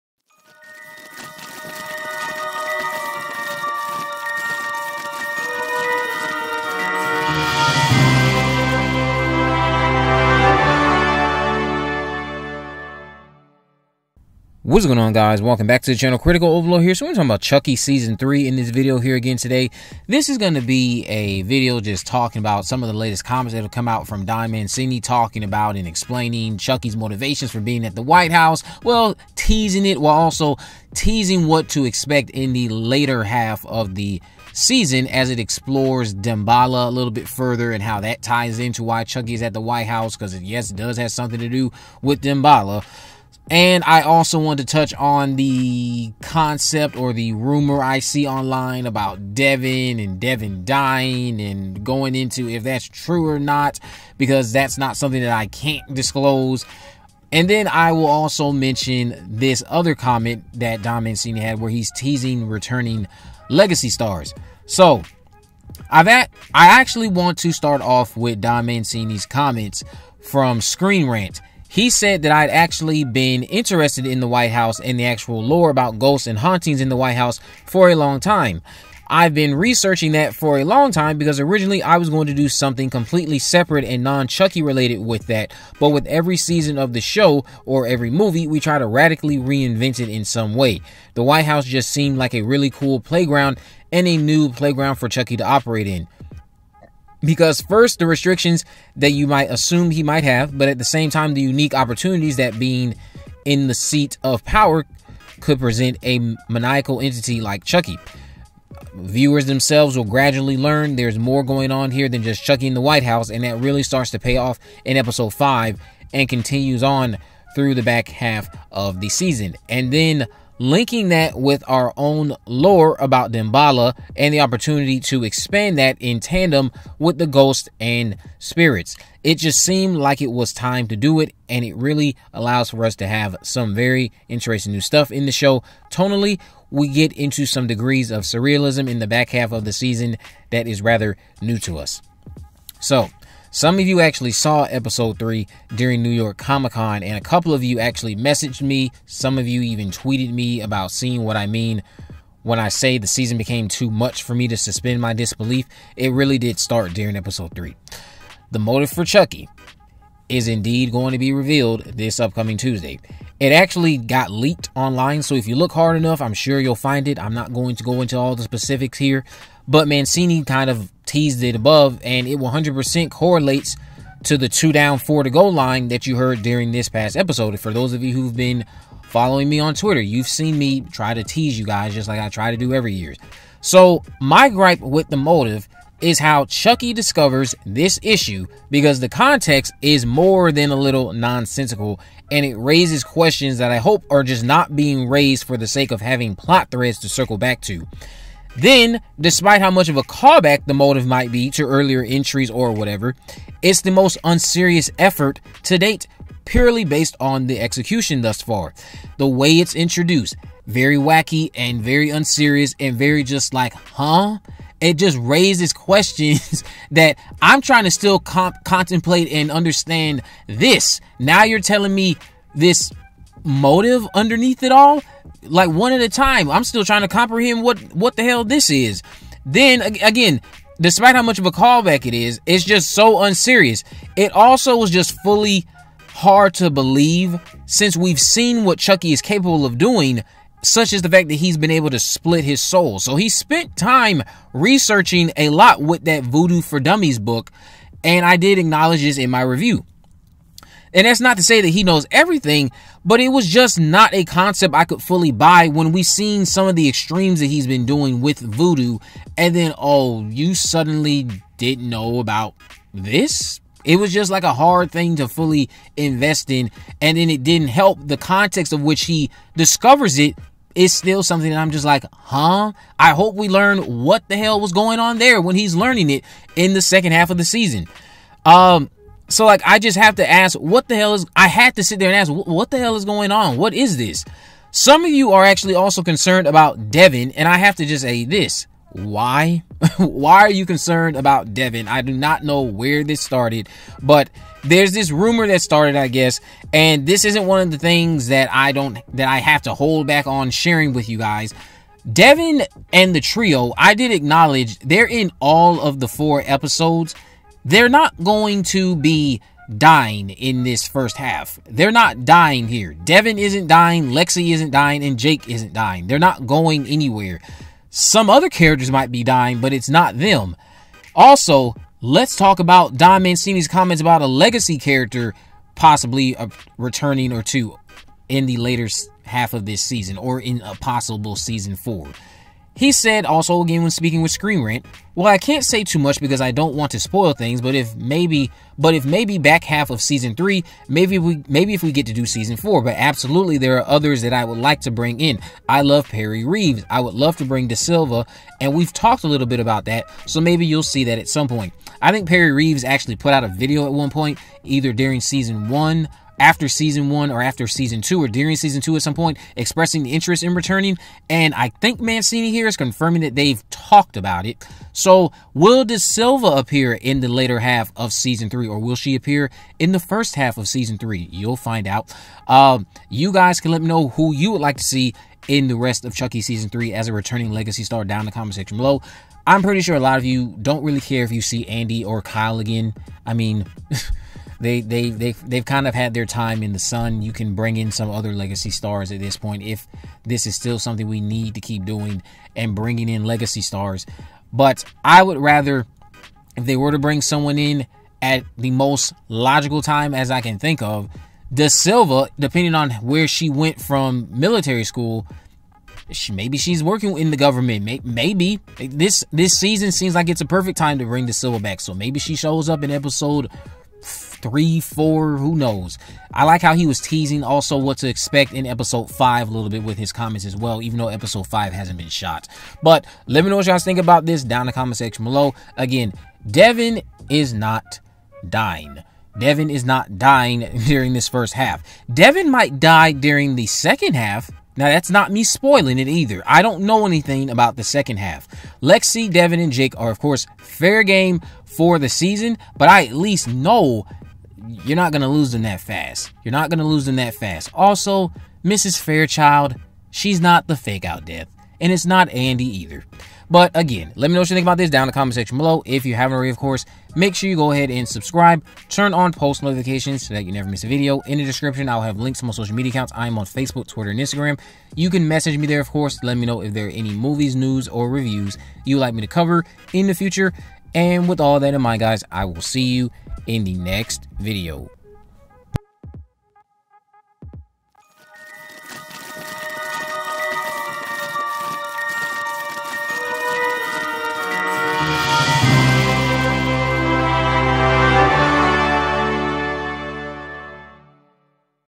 CHUCKY | Season 3 Damballa Motive (Devon Death Rumor, Legacy Return, White House) LEAK
Chucky Season 3 is coming to USA/Syfy according to a recent report from Don Mancini and Deadline. Chucky Season 3 will debut later this Fall it seems and promises to be the scariest season ever. Chucky Season 2 ended this past year, but fans were waiting for the official announcement of Season 3. Recently, the alleged official titles for Chucky's upcoming season have been revealed by spoilertv. Production has paused on Season 3, but new audition tapes have been discovered. Chucky season 3 takes a terrifying turn as the killer doll finds himself inside the White House with America's First Family. The new photos tease Chucky's new life at the White House and introduce Devon Sawa as U.S. President James Collins. Don Mancini has teased a few things about Season 3 and the White House connections.